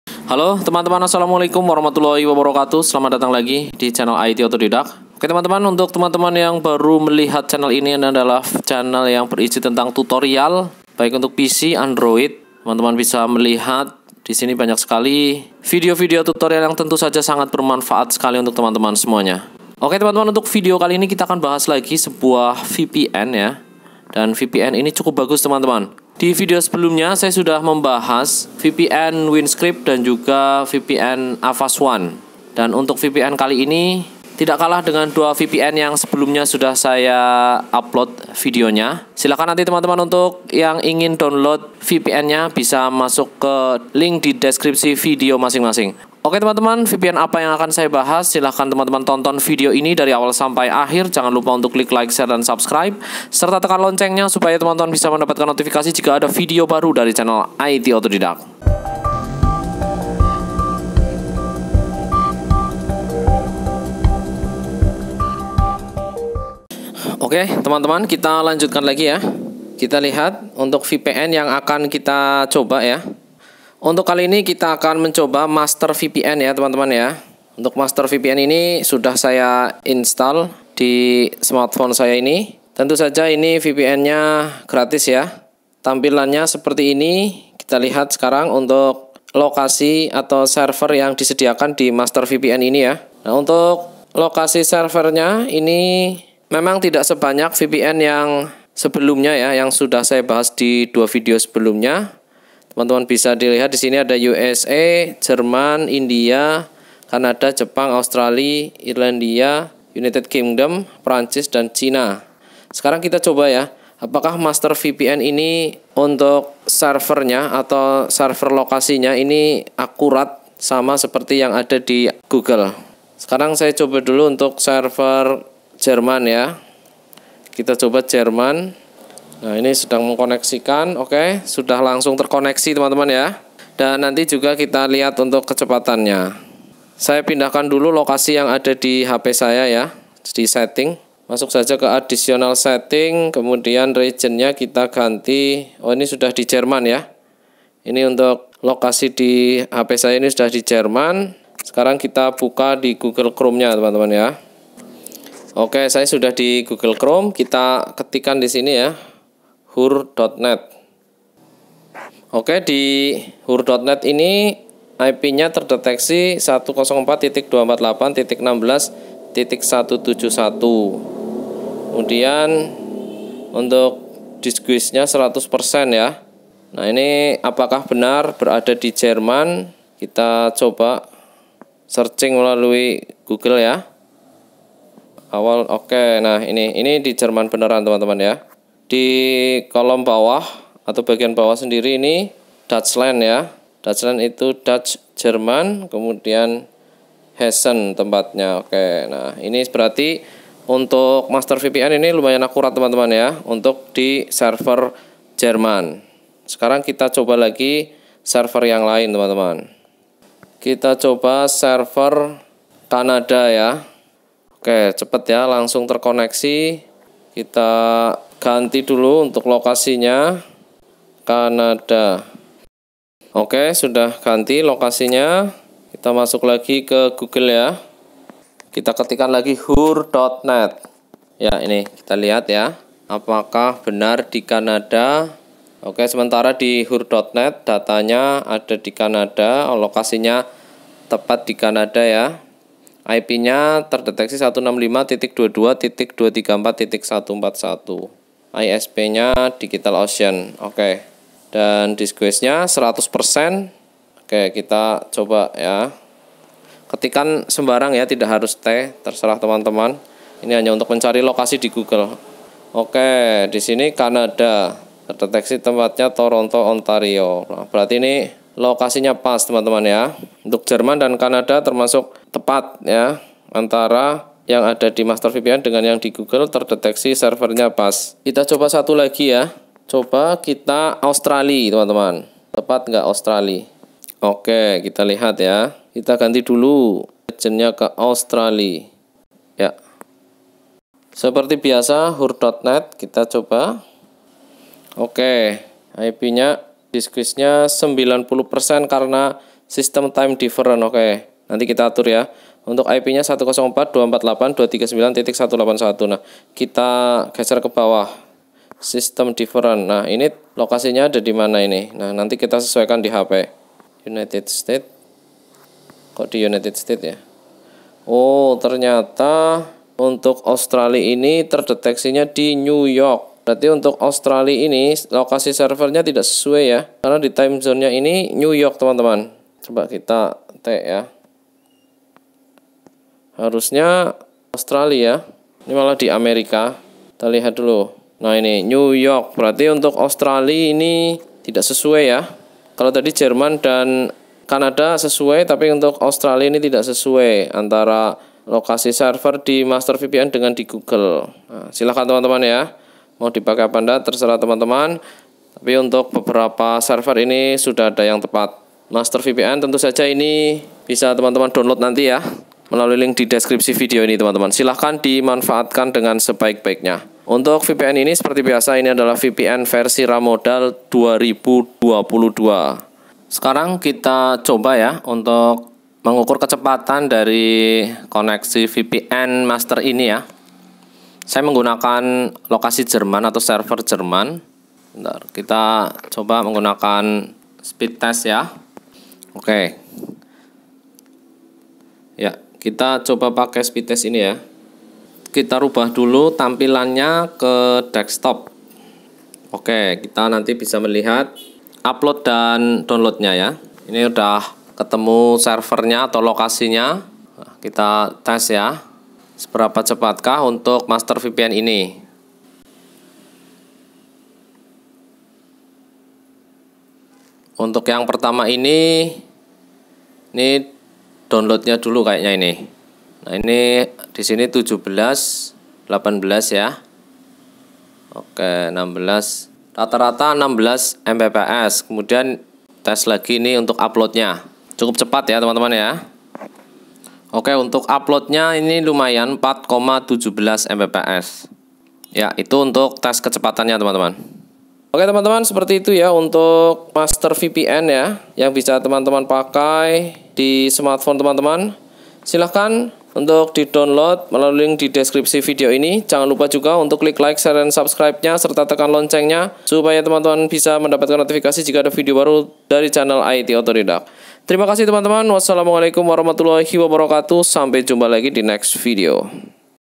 VPN Cepat Untuk Android | VPN Ra modal 2022
Kali ini saya mencoba sebuah vpn. vpn ini bernama master vpn. vpn ini cukup cepat untuk di pakai di smartphone. bagaimana cara menggunakan master vpn. Tonotn video cara menggunakan vpn android tahun 2022 ini dari awal sampai akhir dan jangan lupa untuk klik like share dan subscribe serta tekan loncengnya supaya bisa mendapat notifikasi dari channel @IT_GAMING